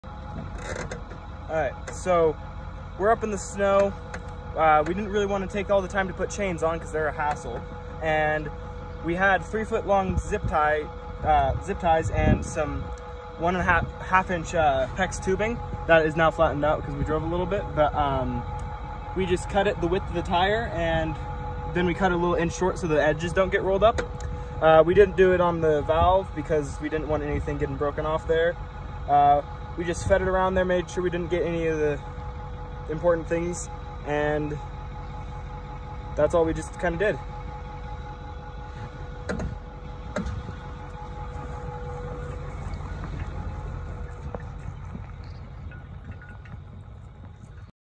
Zip ties as snow chains?
Got stuck a couple times going up a mountain in the snow so we tired out zip ties and 1/2” PEX tube to see if that works. Worked pretty good in a pinch.
Quick and easy to install.